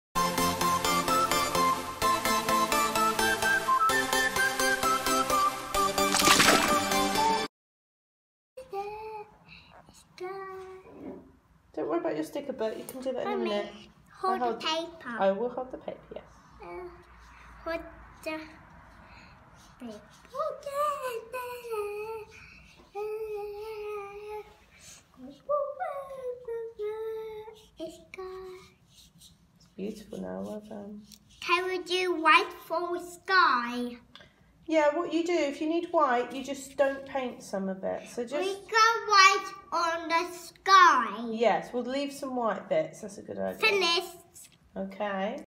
Noah Painting Sky with Tiny Land Watercolours (Wood Stains)
Our Watercolours are great for children, they are food grade and totally washable from fabrics.
Noah wasn't keen on practicing pencil grib with me so I tried this activity -  "Colouring in" with the watercolours.  He was great at keeping within the lines.  I have never done this activity before, as I like to let children express themselves through their own designs.  But the purpose of this activity was to control his pen with purpose and intent.  He did a great job.  I drew the image using a Gruffalo Activity book and Noah painted it using layers.